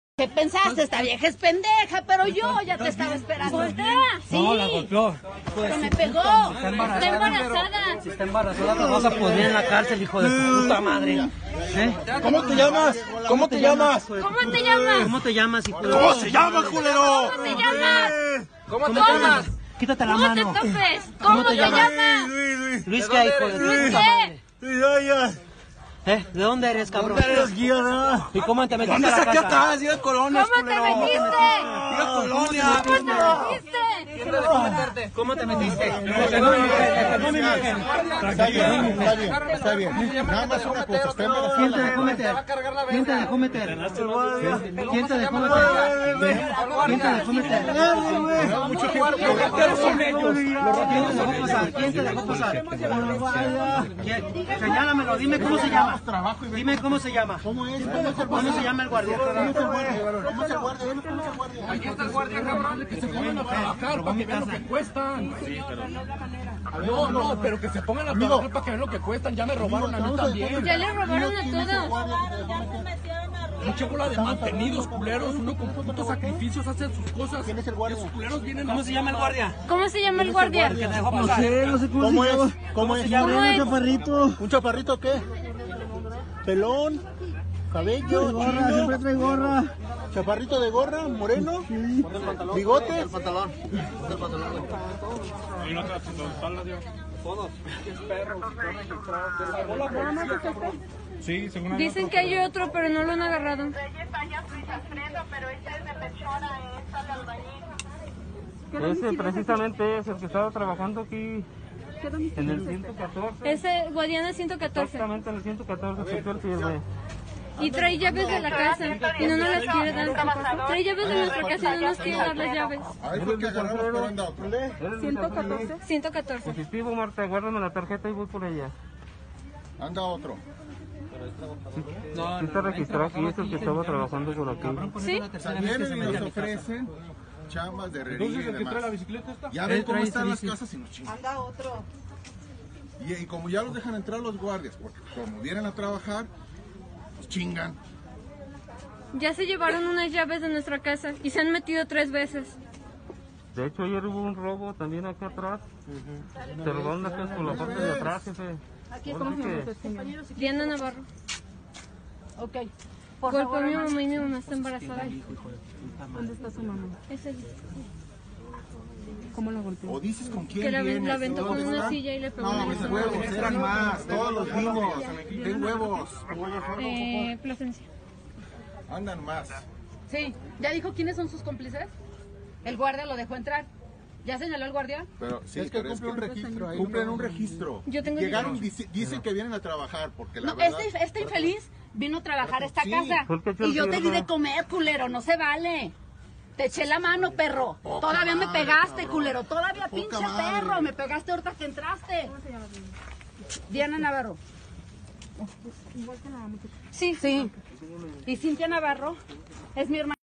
Ciudadanos ya están hartos de la delincuencia y que las autoridades no hagan su trabajo
En esta ocasión los vecinos del fraccionamiento Jardines del Río, en la ciudad de León, atraparon a un amante de lo ajeno, golpeándolo hasta que confesó que los mismos guardias del lugar lo habían dejado pasar.